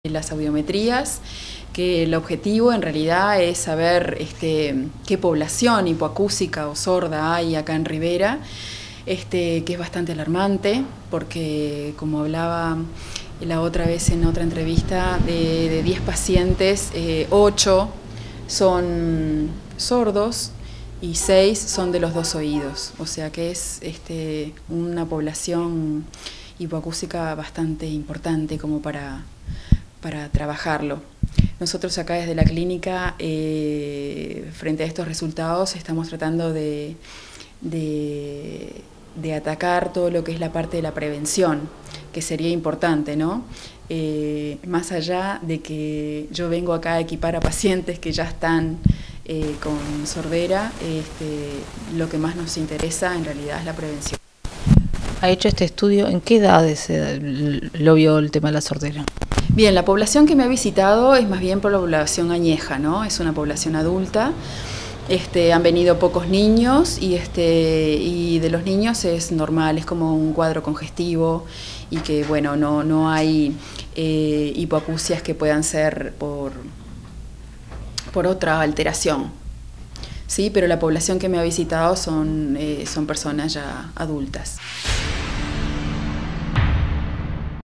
ANDREA ROCA
Audiometrías gratuitas con el objetivo de conocer la cantidad de población sorda en Rivera
Lo cual demuestra cifras alarmantes, de 10 pacientes 8 es sordos